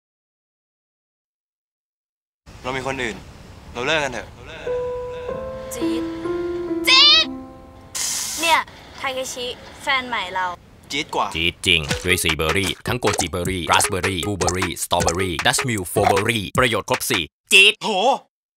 dutchmill 4 berry fuuny thai ad.
over confident guy break up with cute girl directed by Kanin Chandrasma
Production by Phenomena
Agency: Ogilvy & Mather Bkk